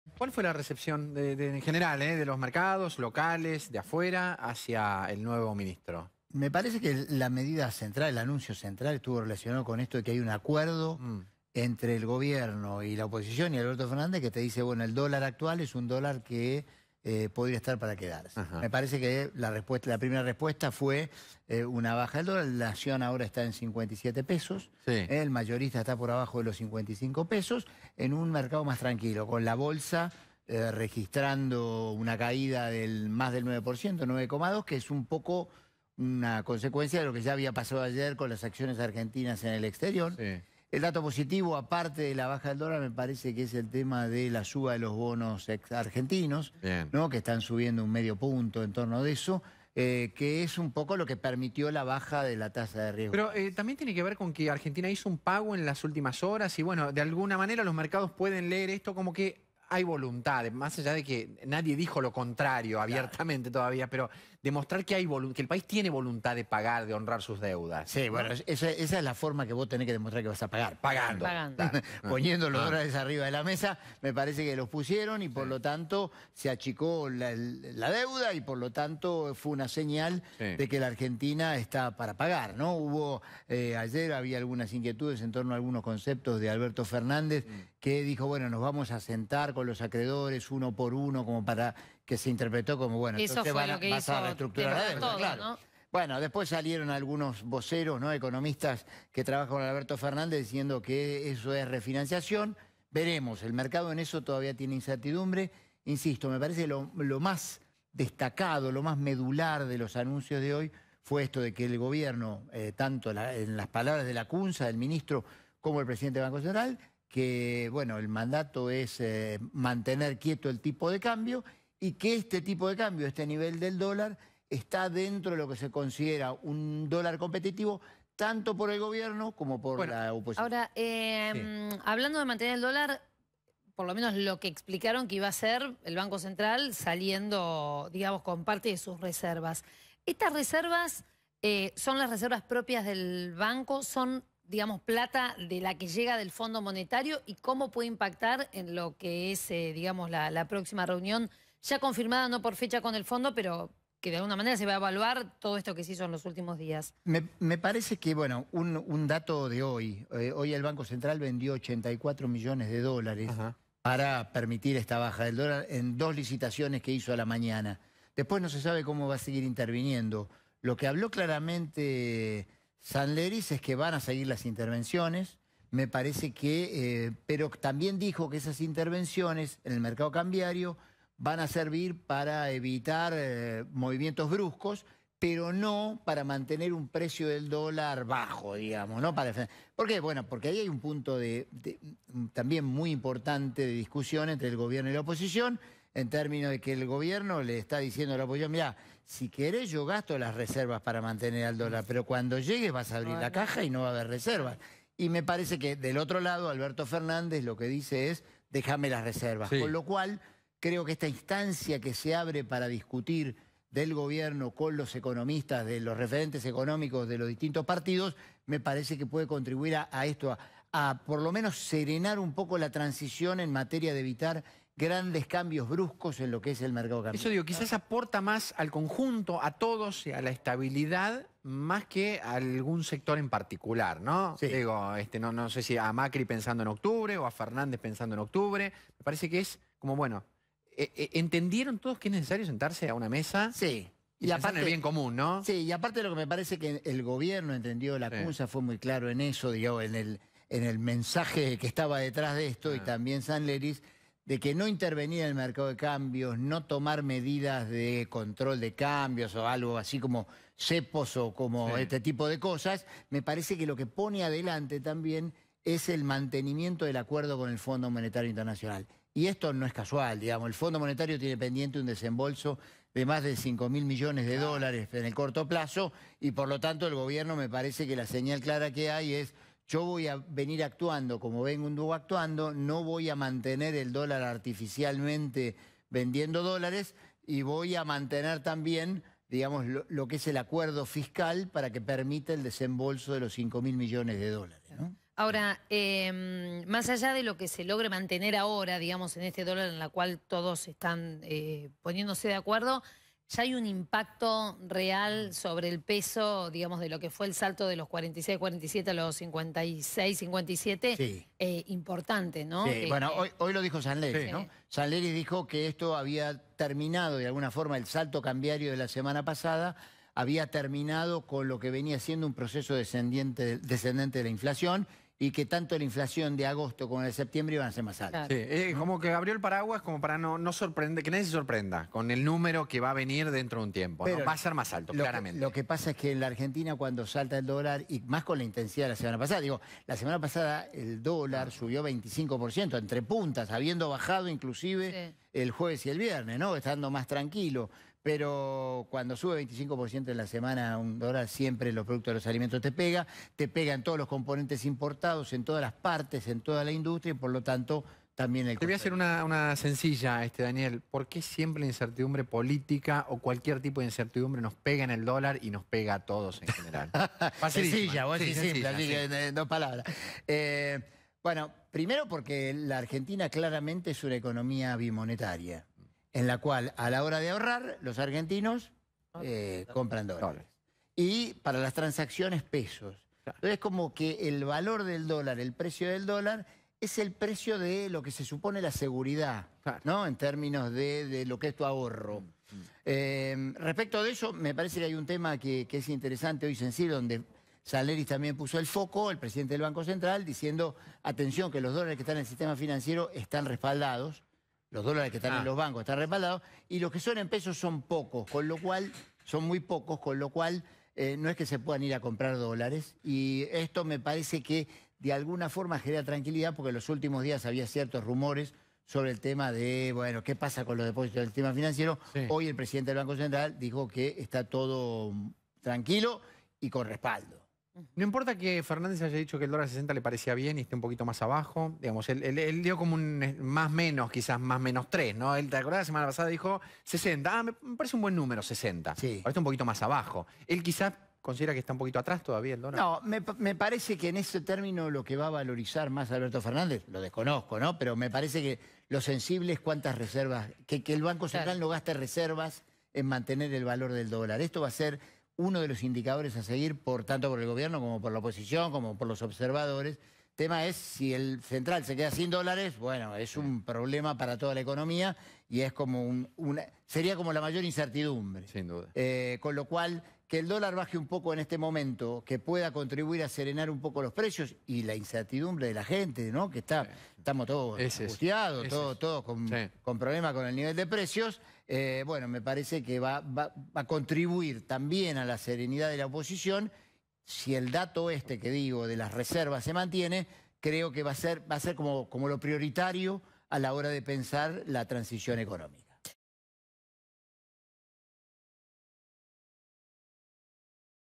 “El mandato es mantener quieto el tipo de cambio”: “la era Lacunza”, según Fernández Canedo
El economista Daniel Fernández Canedo, editor en jefe de la sección economía del diario Clarín, analiza la respuesta de los mercados a la asunción de Hernán Lacunza en el Ministerio de Economía: “El mandato es mantener quieto el tipo de cambio y que este nivel del dólar es un dólar competitivo para el Gobierno y para la oposición”. Las señales de los mercados locales e internacionales.